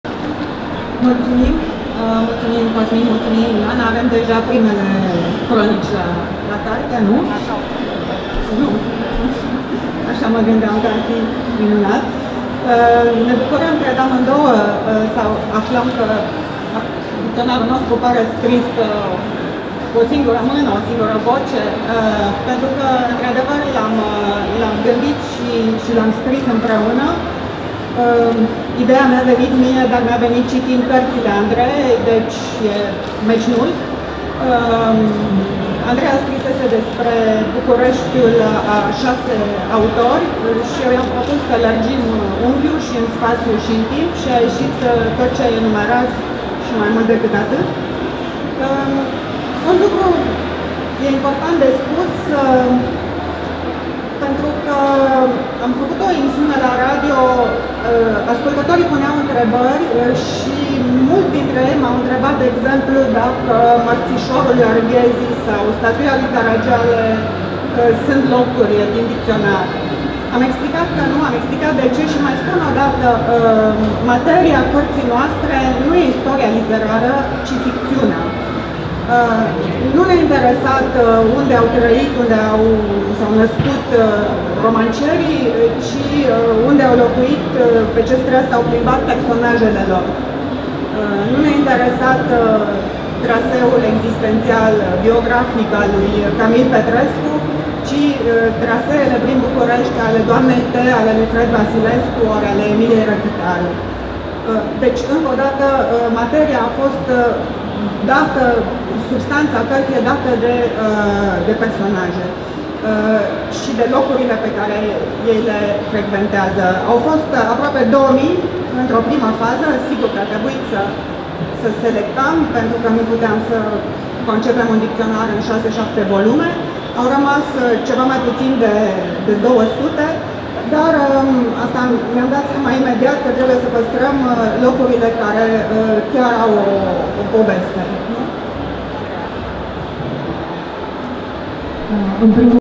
(04) Corina Ciocârlie - Lansare de carte - "Dicționar de locuri literare bucureștene" (Humanitas)
În data de 24 noiembrie 2019 a avut loc lansarea de carte "Dicționar de locuri literare bucureștene" de Corina Ciocârlie, Andreea Răsuceanu în cadrul târgului Gaudeamus 2019.

Vorbitori: Corina Ciocârlie, Andreea Răsuceanu, Ioana Pârvulescu, Cosmin Ciotloș.